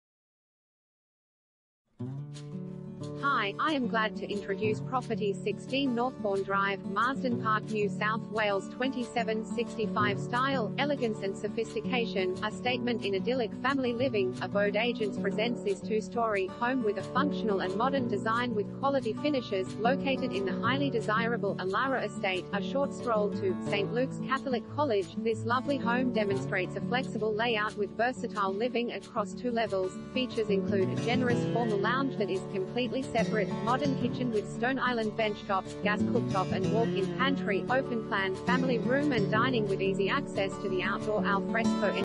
16 Northbourne Drive, Marsden Park NSW 2765 | AuPropertyReport.Com
16 Northbourne Drive, Marsden Park NSW 2765
Property Type: house
Bedrooms: 4
Bathrooms: 4
Car Space: 2
STYLE, ELEGANCE &amp; SOPHISTICATION